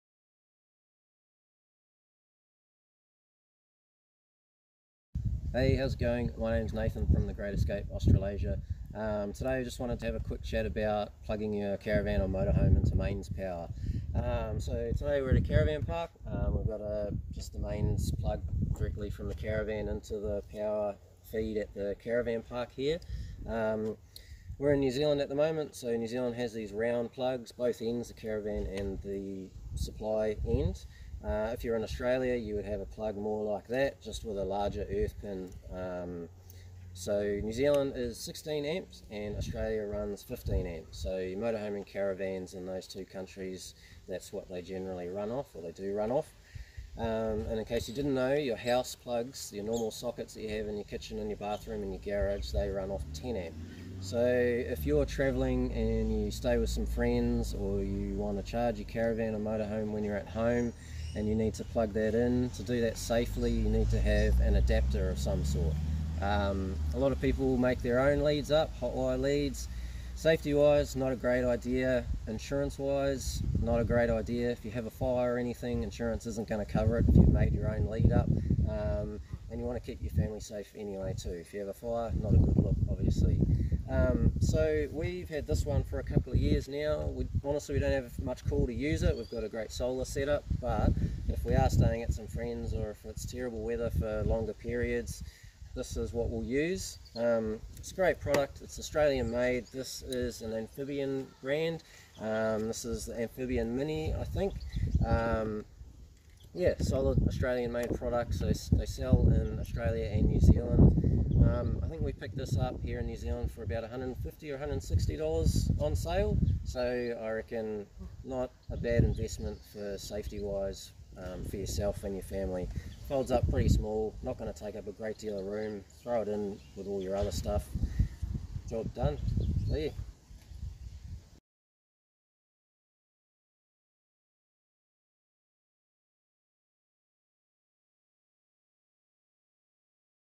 Ampfibian Mini - Plus Power Adapter
Do you need to take a power adapter on your travels?

The short answer is; yes, yes you do. If you think there's any chance at all that you'll be plugging your RV into a power supply outside of a caravan park (think your mates place, a park over property, or even your own driveway...) then a power adaptor is a must. And whatever you do don't listen to that bloke at the campsite who says you can just hotwire your own one 😬🔥

We bought a Mini-Plus adapter from Ampfibian here in NZ right after we got our caravan. They're an Aussie company with products to suit both the Aus & NZ market and it'll only set you back about $100 - $150 or so (like most things they're cheaper in Aus than NZ)

Nathan explains in this video why a power adapter is a good idea, so if you're like me and sometimes get a bit confused between your amps, volts and wattages then check it out 😉